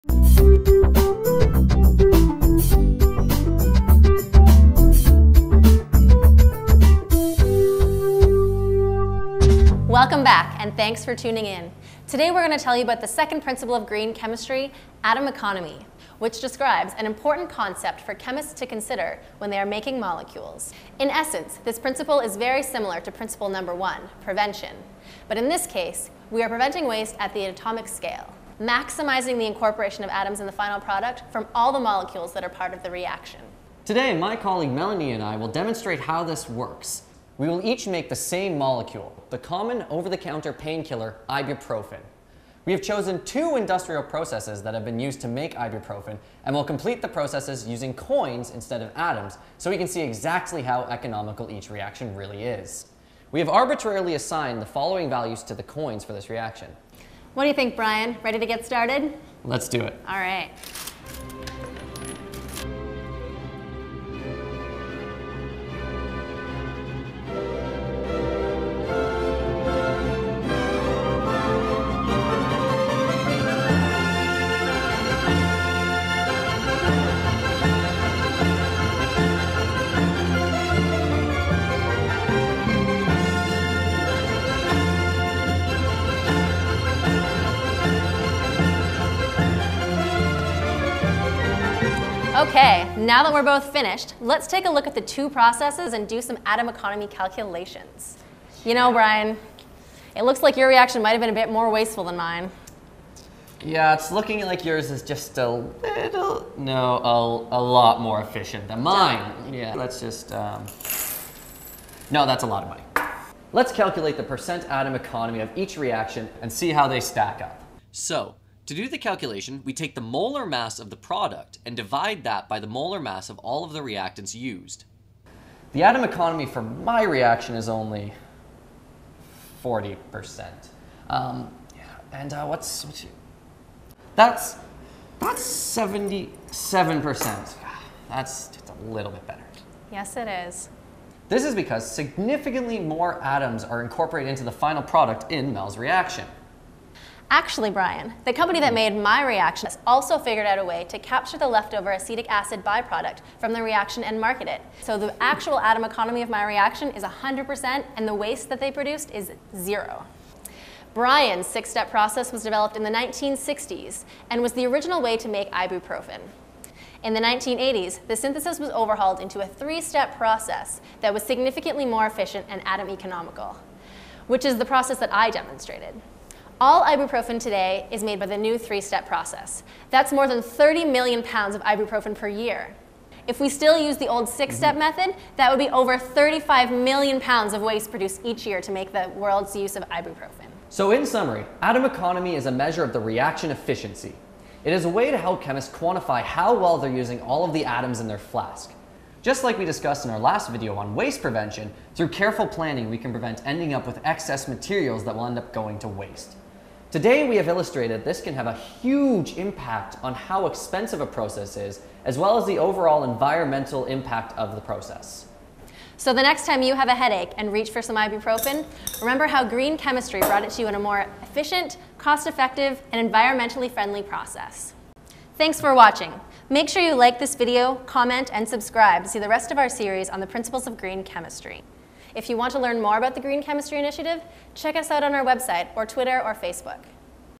Atom Economy - Green Chemistry Principle #2
The Green Chemistry Initiative explains atom economy by looking at the current way ibuprofen - the common painkiller - is made, and comparing it with the old, less efficient, method.